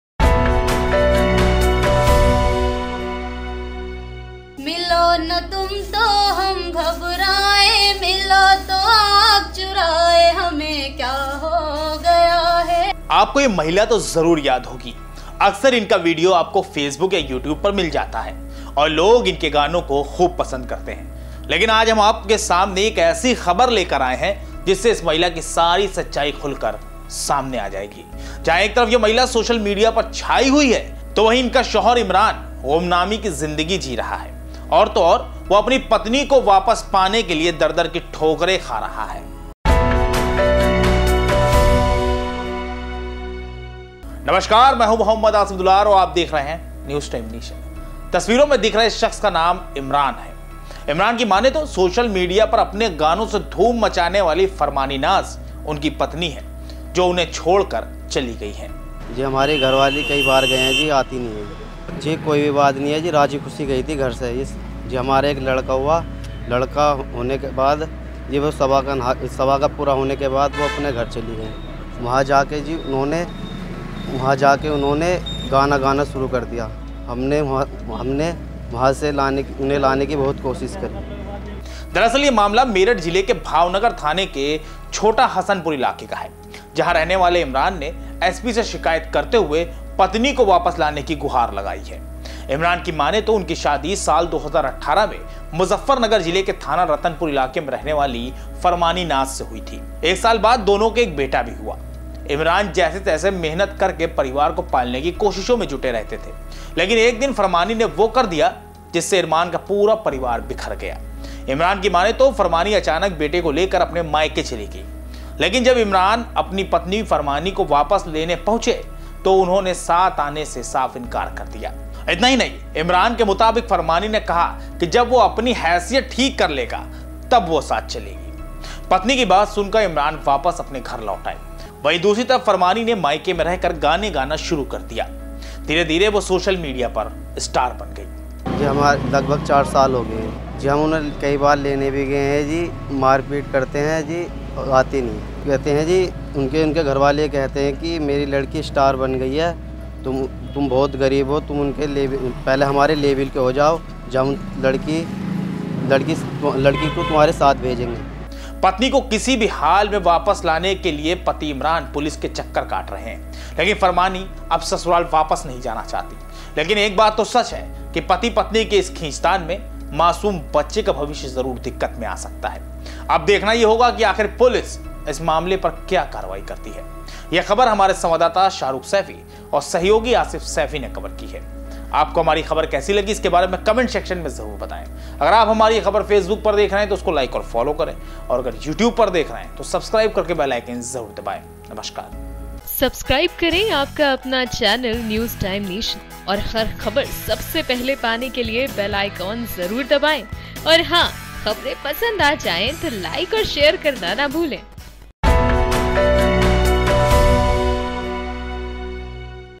Meerut : गायक अरमानी नाज का पति खा रहा है ठोकरें ! || News Time Nation
आपको ये महिला तो जरुर याद होगी...अक्सर इनका वीडियो आपको फेसबुक या यूट्यूब पर मिल जाता है...और लोग इनके गानों को पसंद भी खूब करते हैं...लेकिन आज हम आपके सामने एक ऐसी खबर लेकर आए हैं...जिससे इस महिला की सारी सच्चाई खुलकर सामे आ जाएगी...जहां एक तरफ ये महिला सोशल मीडिया पर छाई हुई है तो वहीं इनका शौहर इमरान गुमनामी की जिंदगी जी रहा है...और तो और वो अपनी पत्नी की वापस पाने के लिए दर दर की ठोकरे खा रहा है...